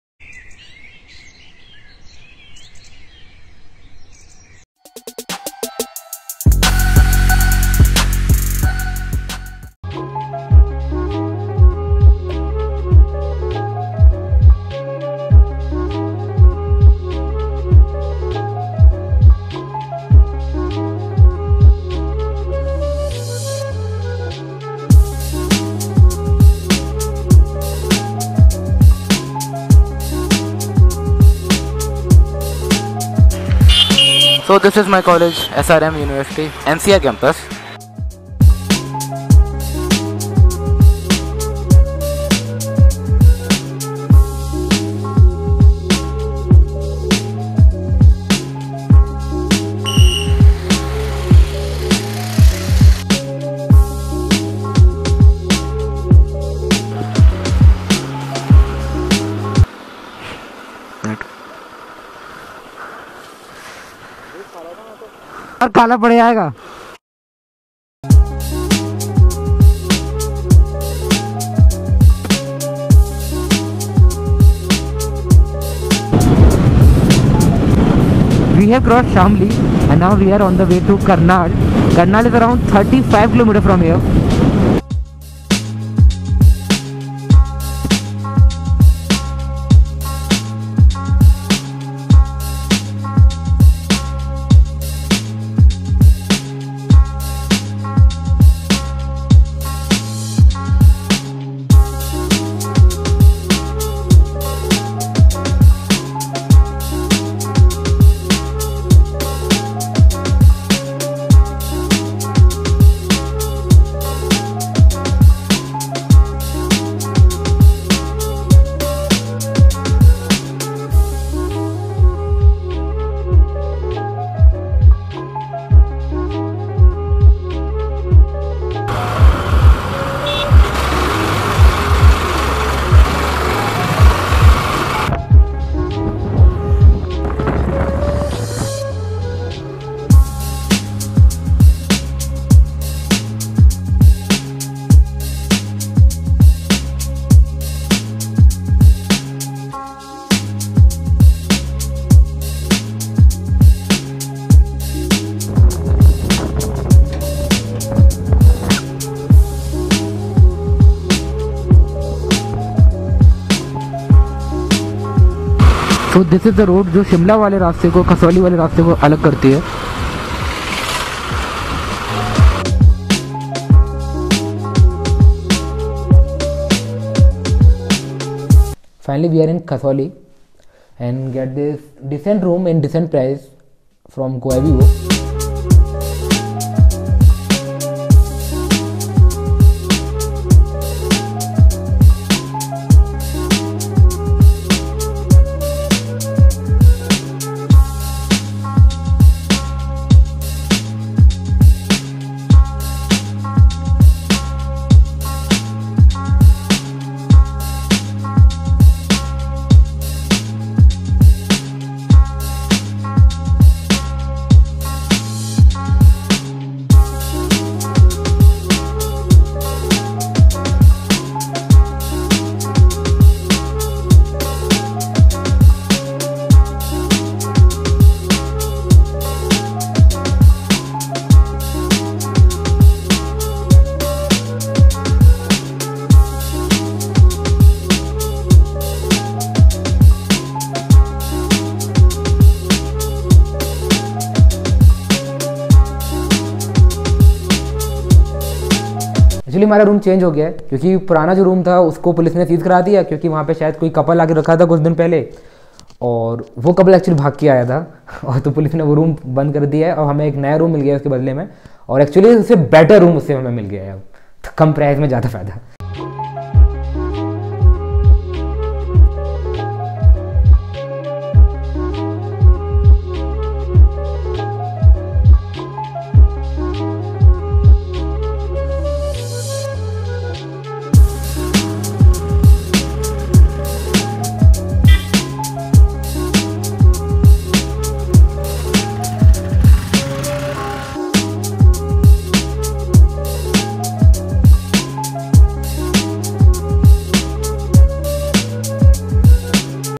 KASAULI ROADTRIP
My first travel vlog Ghaziabad to Kasauli ride, it’s part (1) of my journey.
second part will release soon.